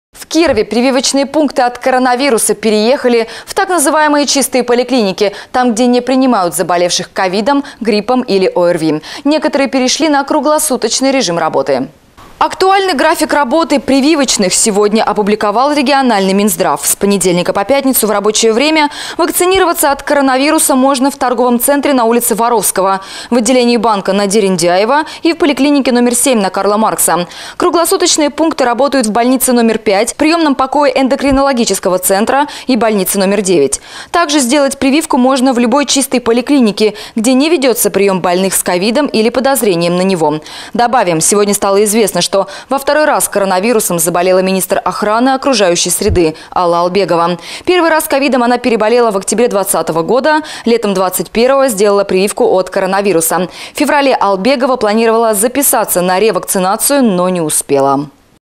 Албегова заболела коронавирусом 2 раз Новости Кирова 01 02 2022
В Кирове прививочные пункты от коронавируса переехали в так называемые «чистые» поликлиники, там где не принимают заболевших Ковидом, гриппом или ОРВИ. Некоторые перешли на круглосуточный режим работы